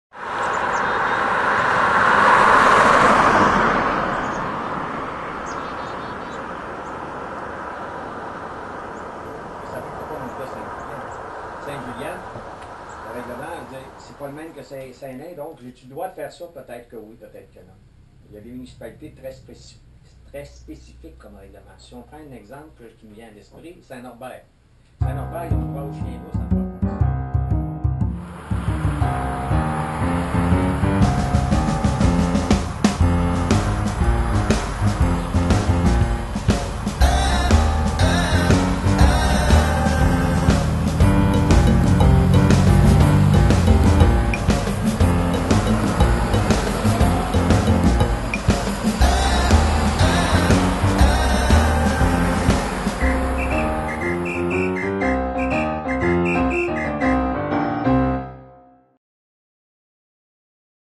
Escouade des contrôleurs Inspecteur Canin
QG Inspecteur Canin, 8h30, Saint-Lin-des-Laurentides.

Après un petit briefing matinal, c'est le départ de l'escouade
des contrôleurs Inspecteur Canin.